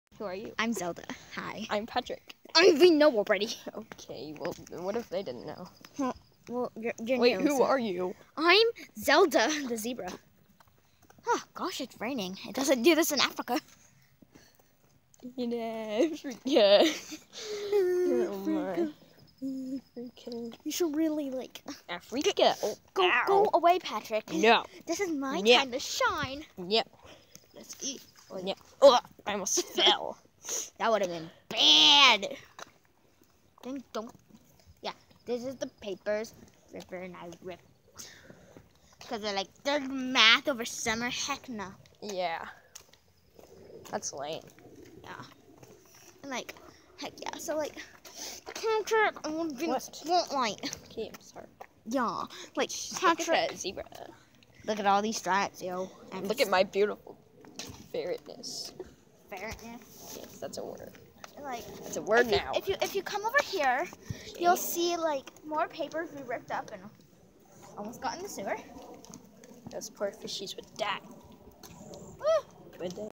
A Little Walk With My Friendee
Short walk. When we normally walk, we record. But yea, so. hey! I'm not dead!. sadly.

Featuring:

Patrick (Signature Black Footed Ferret)
Zelda (Signature Endangered Cape Mountain Zebra)
Google (My fren)

Patrick is new and Zelda belongs to my friend (Zelda's a girl btw had to say that). My friend is Google and I'm Siri. It's our "code names" I guess you could say, but that'll come up in other videos, so yea.

Țhåňķş føř wąťçhīňğ, býə!